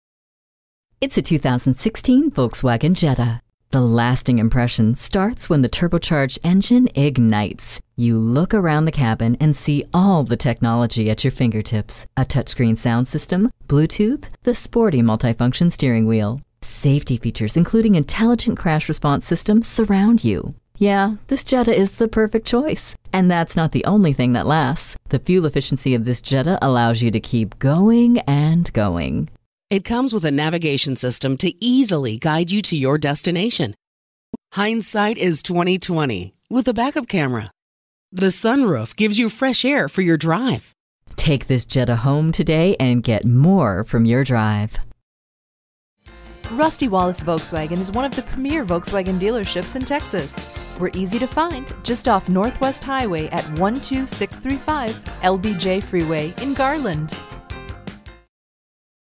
New 2016 Volkswagen Jetta Dallas TX Garland, TX V160822 - SOLD
Year: 2016
Make: Volkswagen
Model: Jetta
Trim: 1.8T SEL
Engine: 4 Cyl - 1.8 L
Transmission: Automatic
Color: Platinum Gray Metallic
Interior: Black
Stock #: V160822
VIN: 3VWL07AJ9GM391120
This superb 2016 Volkswagen Jetta is the rare family vehicle you are searching for. Climb into this durable Jetta and drive right over the norm.

Address:
12635 LBJ Freeway